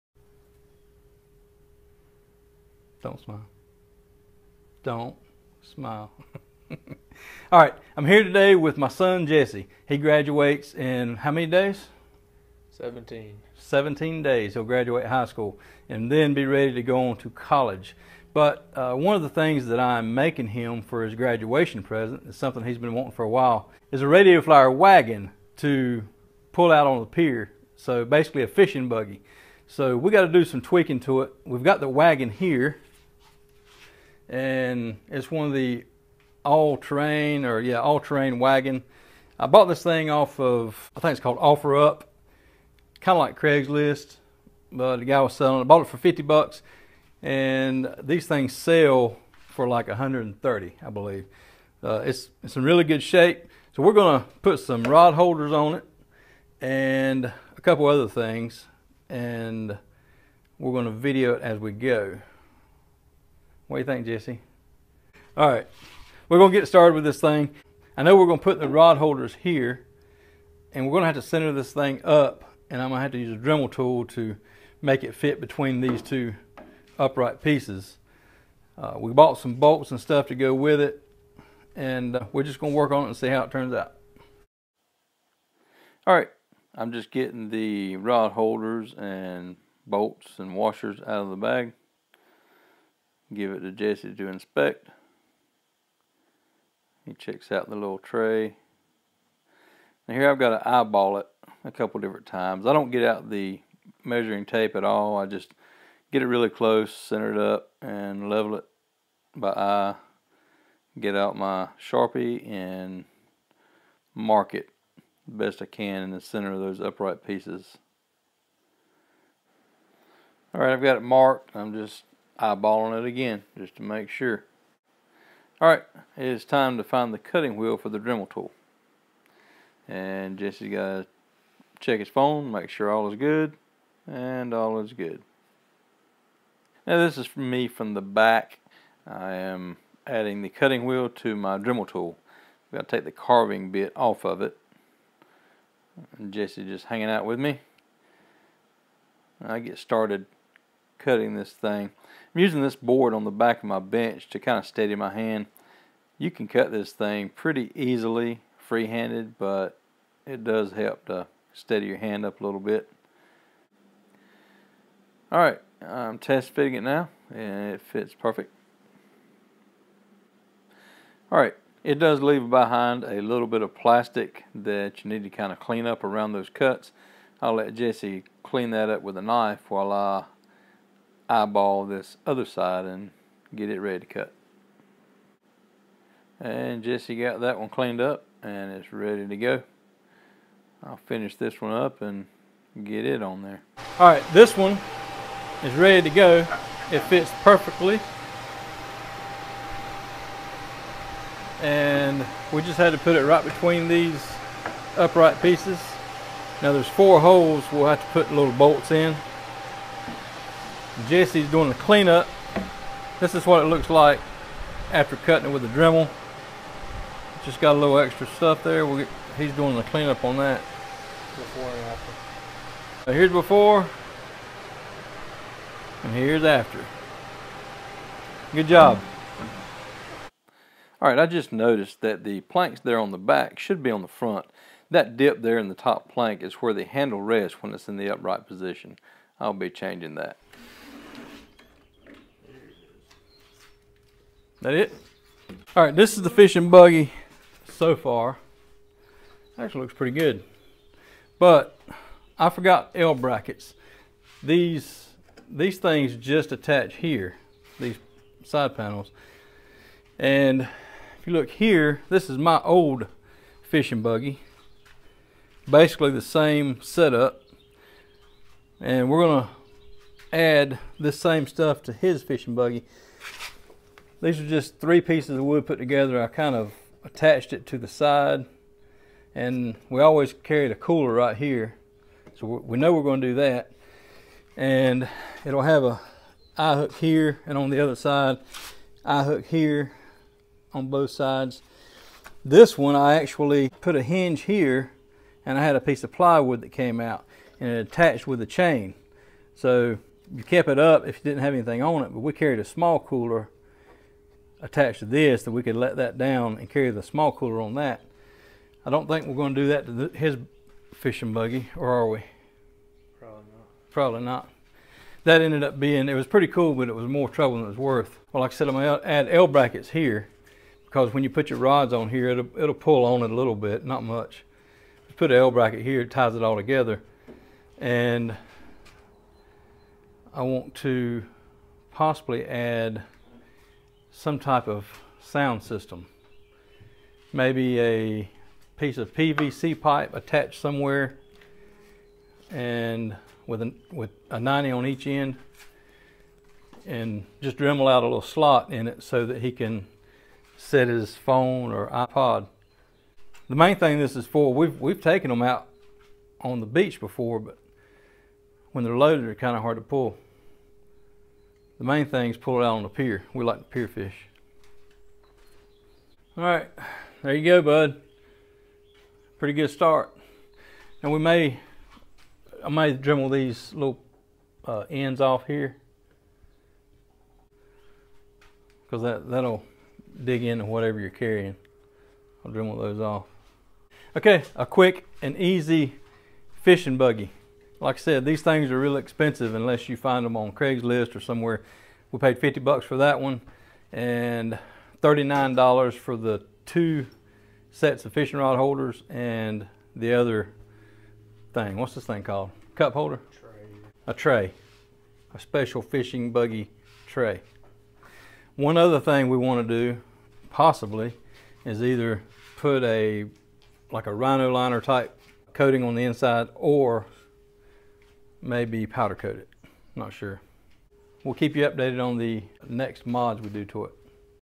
Radio Flyer Fishing Buggy
Some of our favorite times have been spent fishing on the pier at Oak Island North Carolina. Jesse, our oldest son, wants to carry on our pier fishing traditions and he has been wanting a Radio Flyer fishing buggy just like the one I made several years ago. As part of his high school graduation present, I told him that I would make him a fishing buggy like he wants but, he has to work on this project with me. Check out this fishing buggy and be on the lookout for our updates as we tweak this thing periodically until it is completed.

Mailing Address
Jeff Furr